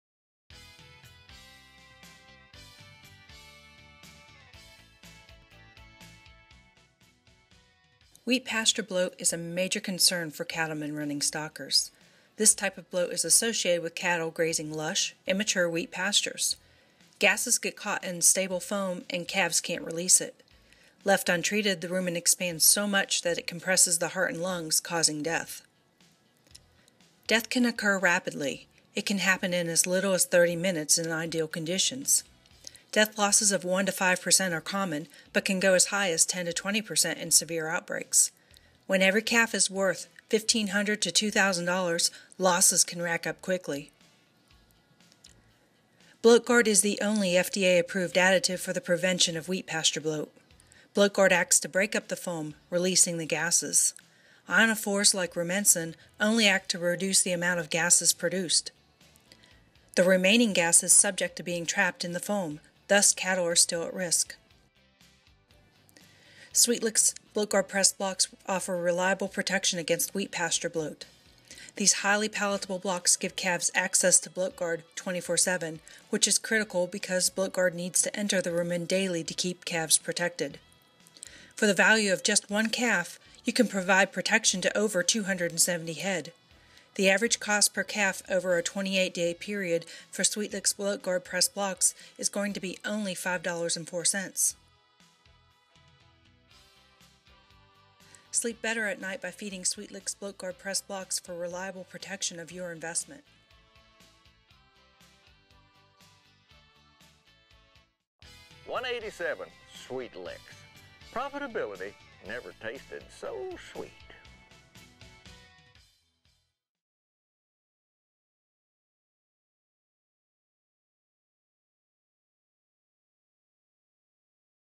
SWEETLIX Bloat Guard Pressed Blocks
Wheat pasture bloat is a major concern for cattlemen running stockers. Protect your investment with SWEETLIX Bloat Guard pressed blocks.